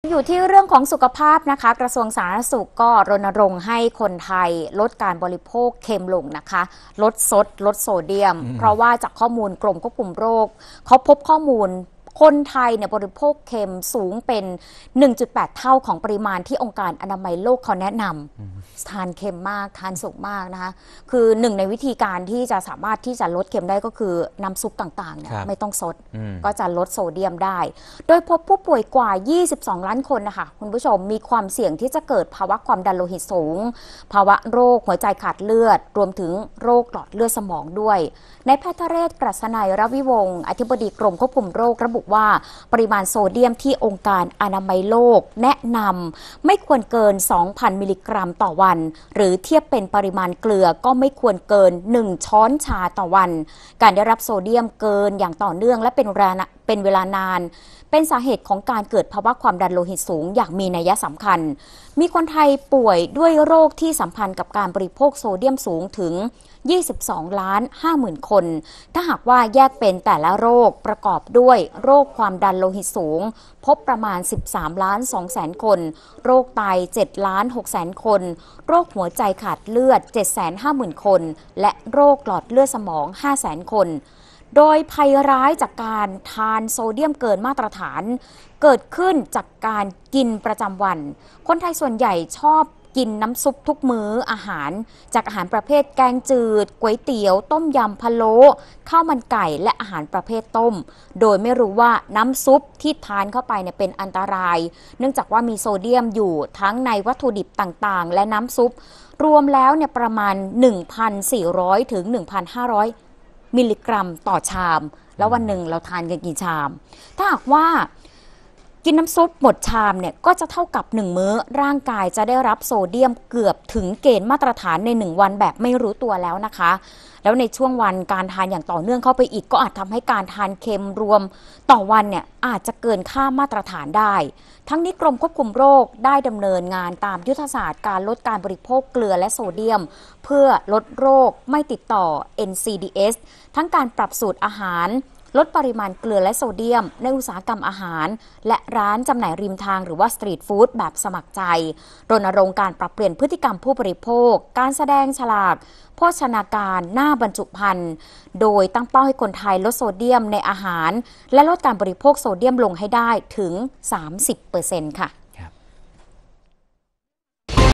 คนไทยบริโภคเค็มสูง 1.8 เท่าของปริมาณที่ WHO แนะนำ ข่าวเช้า วันที่ 8 กุมภาพันธ์ 2566
กรมควบคุมโรค เปิดเผยข้อมูลคนไทยบริโภคเค็มสูงเป็น 1.8 เท่าของปริมาณที่องค์การอนามัยโลกแนะนำ โดยพบผู้ป่วยกว่า 22 ล้านคนมีความเสี่ยงเกิดภาวะความดันโลหิตสูง โรคหัวใจขาดเลือด  โรคหลอดเลือดสมอง